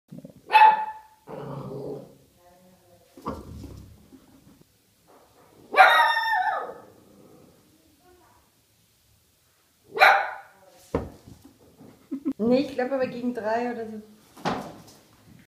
Italian Greyhound | Milo scared of a fake horse
Funny clip of my dog.